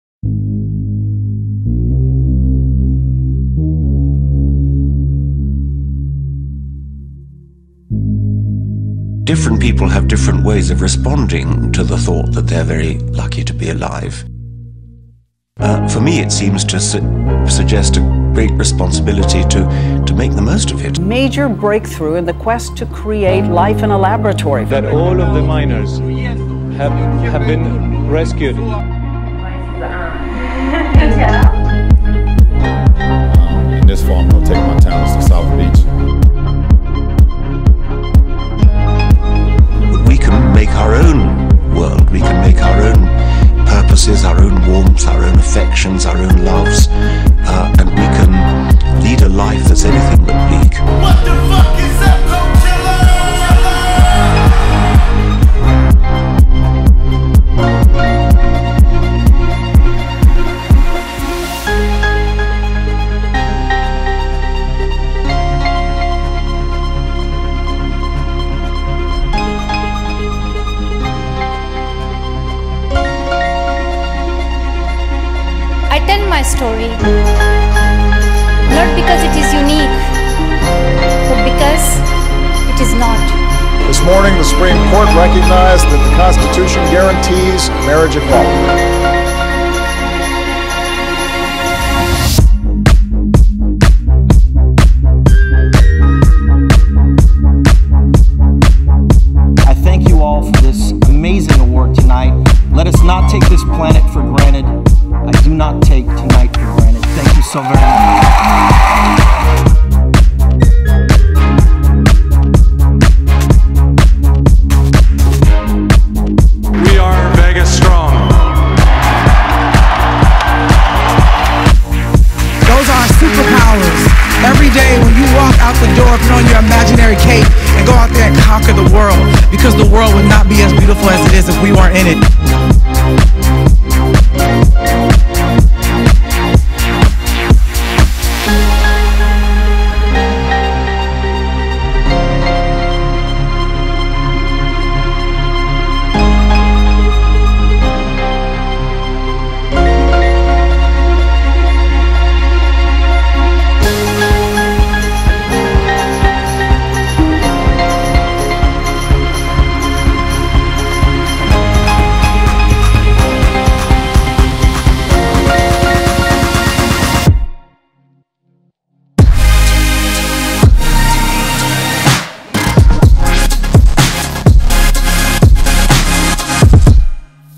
DECADENCE 2019 LOUIS THE CHILD BALL DROP DECADE RECAP
Louis The Child NYE 2019 ball drop visual highlighting the some of the most monumental moments of the last decade.